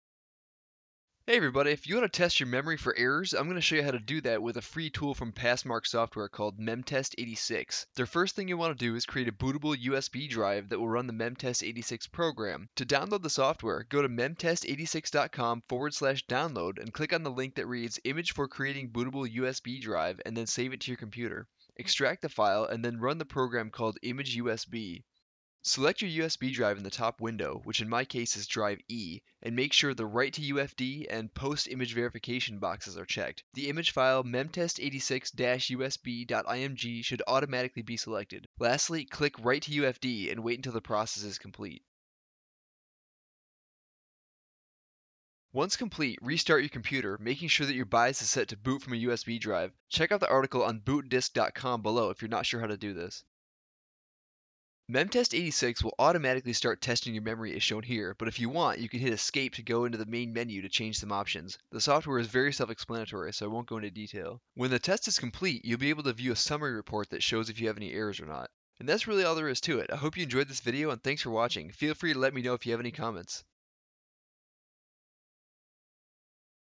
Test RAM with MemTest86, now with UEFI support!
Here's how to test your RAM for errors with MemTest86, and how to make a bootable USB drive to do so.